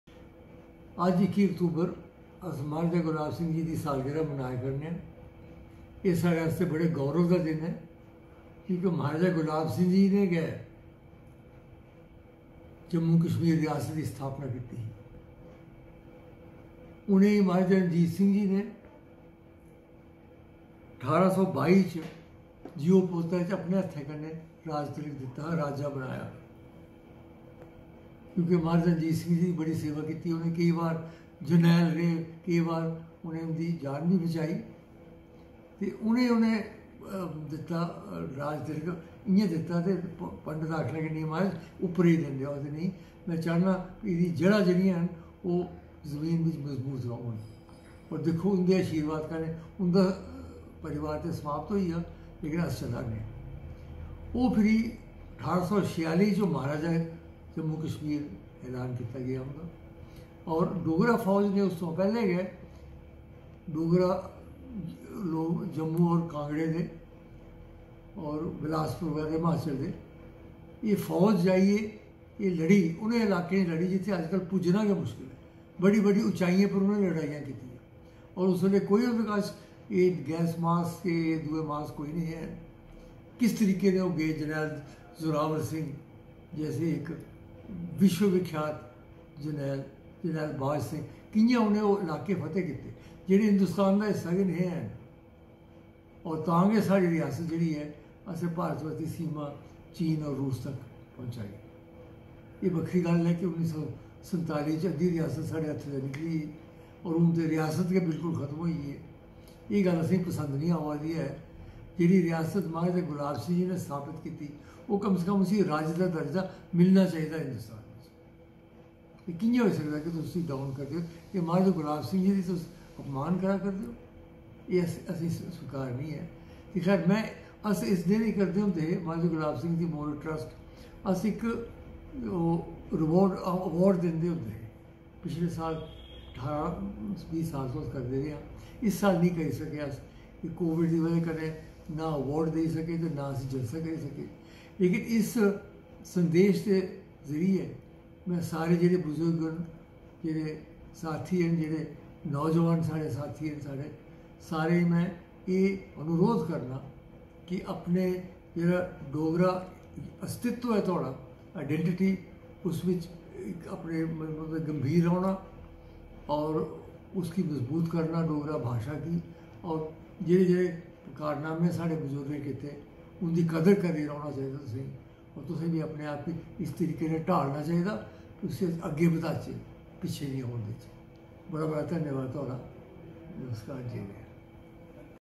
Maharaja Gulab Singh | Birth Anniversary | Tribute by Dr. Karan Singh | Jammu and Kashmir | Dogra
Remembering Maharaja Gulab Singh,  Founder of the State of Jammu Kashmir on his 228th Birth Anniversary. An incredible man, the most talked about personality of 19th century all over the World.
Dr Karan Singh’s tribute toMaharaja Gulab Singh on his birth anniversary!